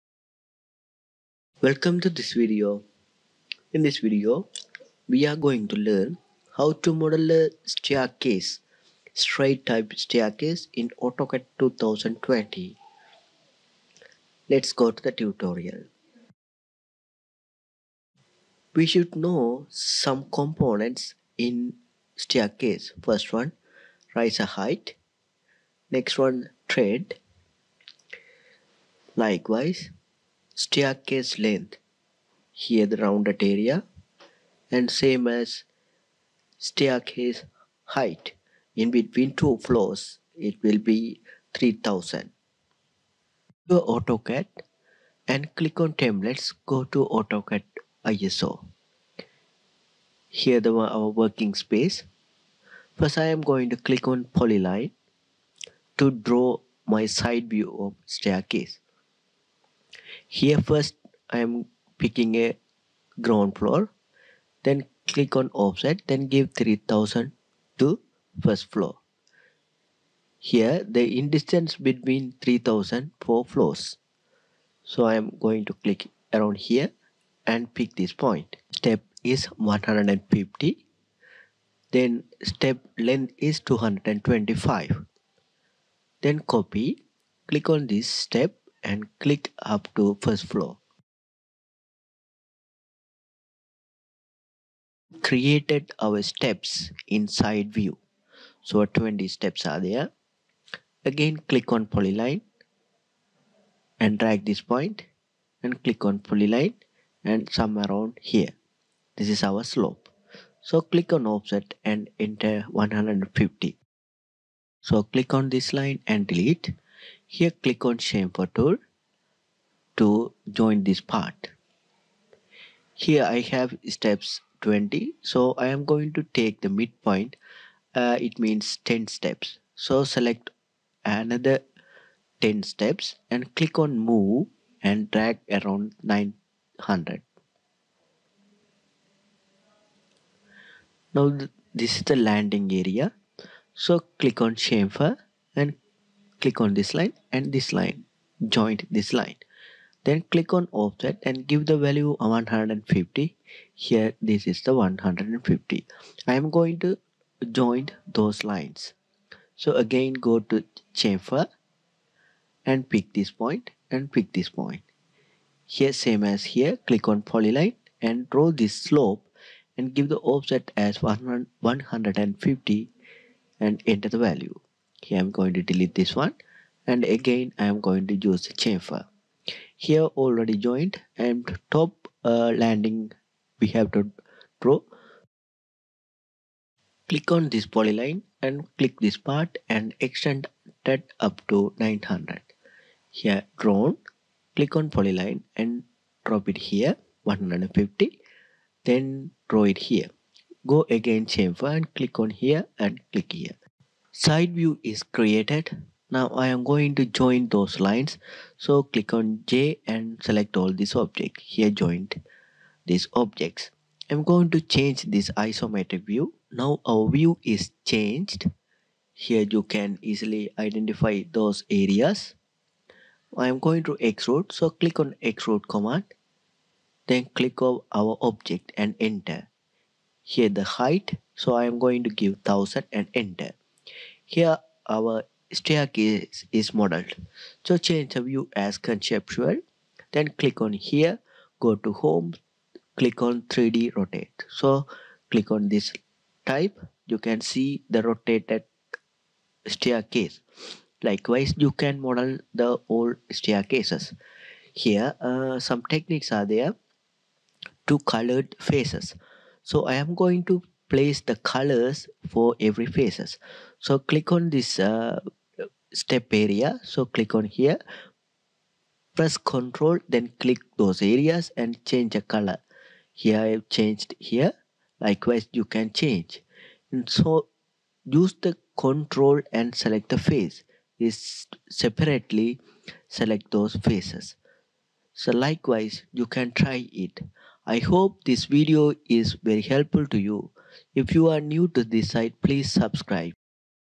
How to design straight staircase in AutoCAD 2020
How to Design Staircase in AutoCAD.
Explained How to Design a 3D Staircase in AutoCAD.
I AM USING
AutoCAD 2020 Beginners Tutorial Series.